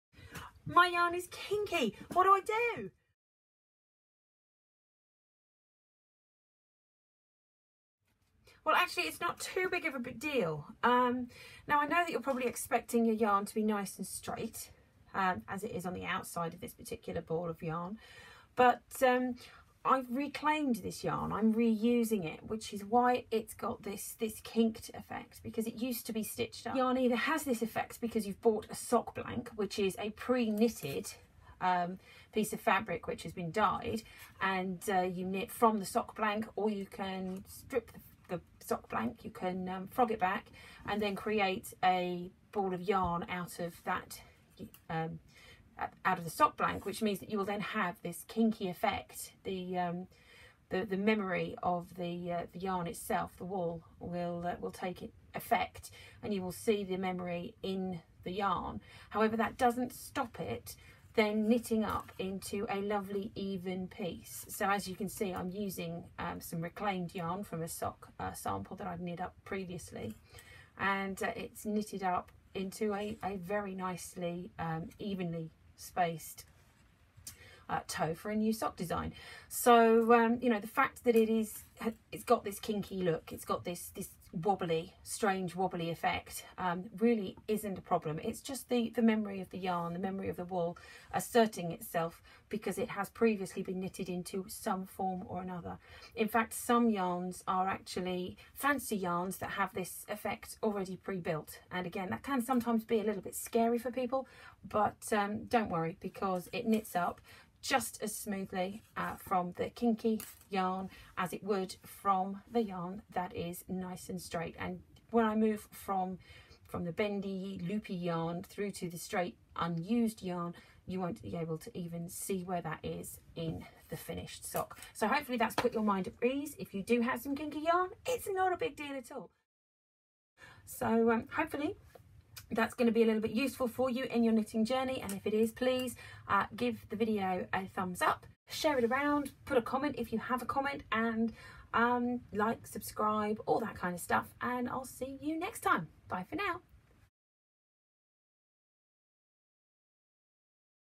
Does Yarn with Kinks Matter?
If you work with wibbly wobbly (not timey wimey) yarn will effect your finished object?